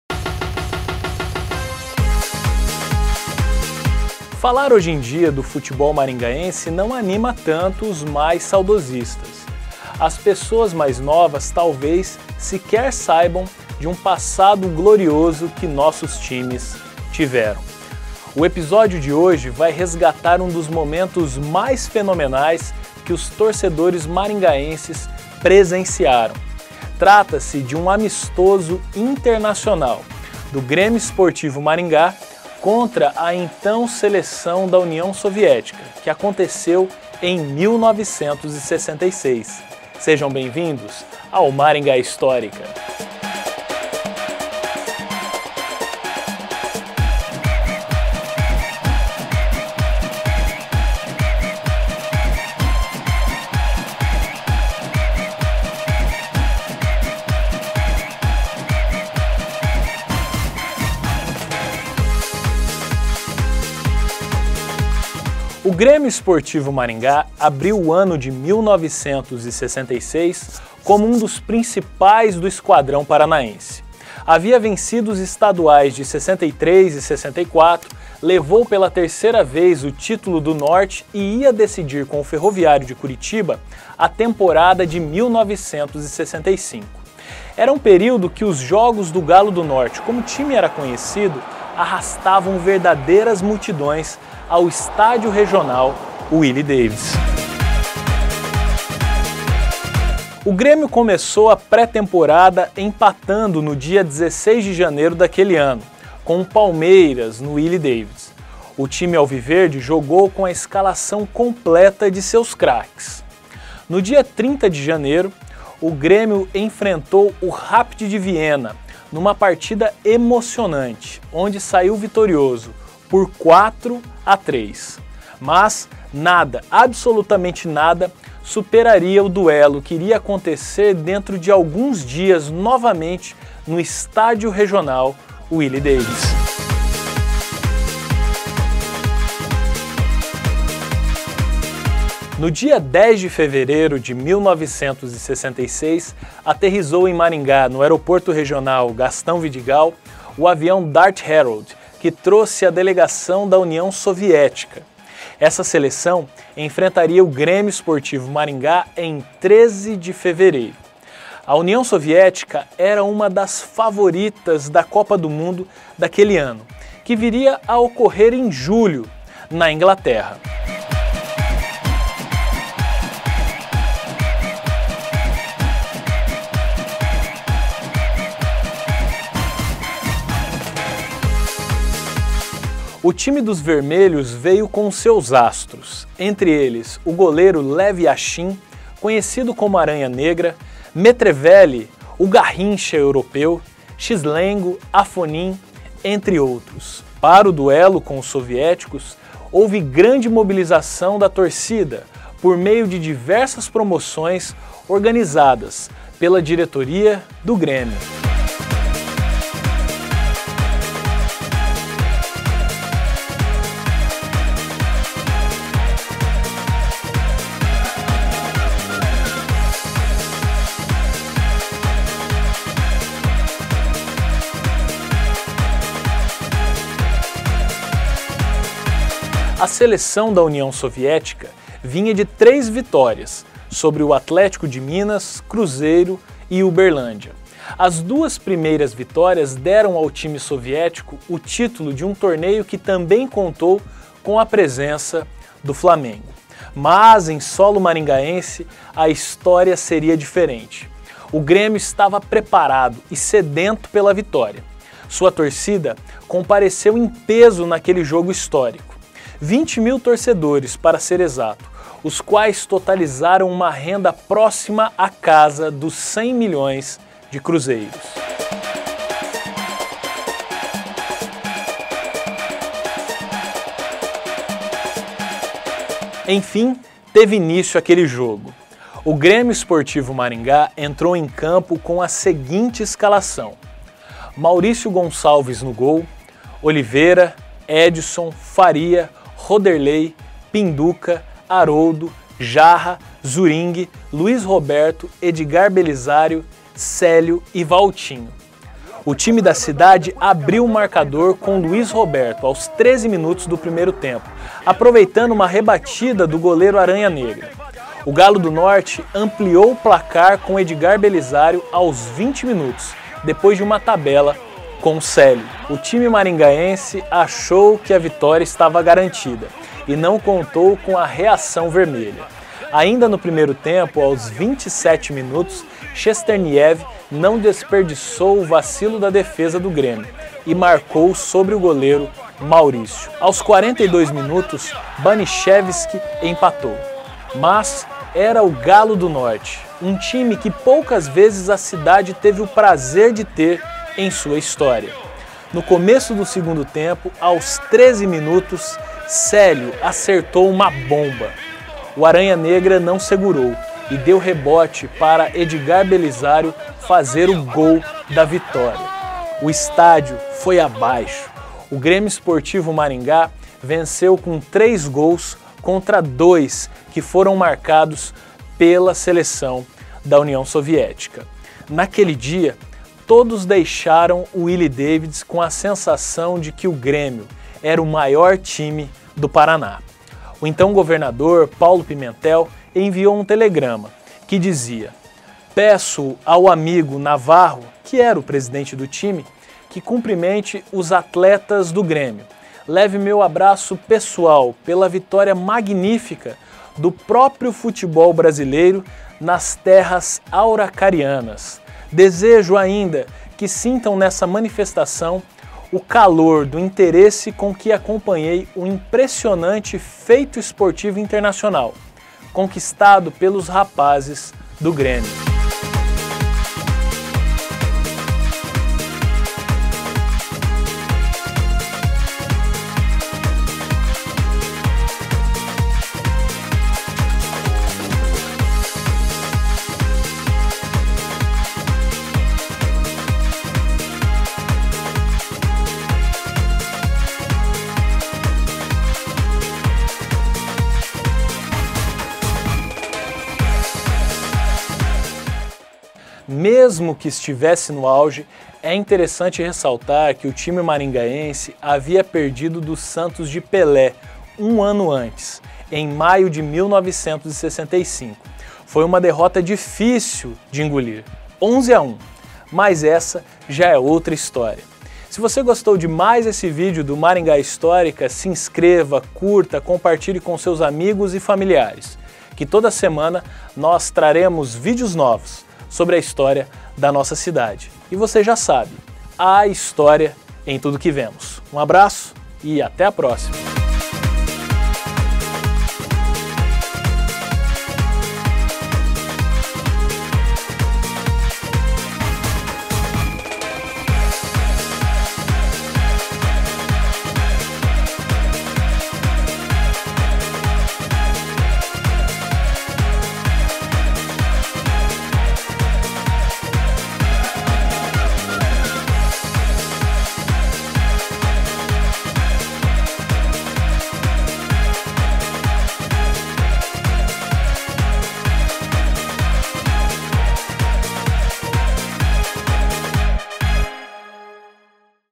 Soviéticos em campos maringaenses
No dia 13 de fevereiro de 1966, um jogo jamais imaginado ocorreu no Estádio Regional Willie Davids. O Grêmio Esportivo Maringá (GEM), que havia conquistado o título estadual nos anos de 1963 e 1964, enfrentou a seleção da União Soviética (CCCP), que era uma das favoritas para a Copa do Mundo daquele ano, que ocorreria em julho na Inglaterra.

Relembre o passado glorioso do futebol maringaense.

Seja membro do clube do Maringá Histórica
(clique aqui embaixo e saiba mais no ícone "Seja Membro").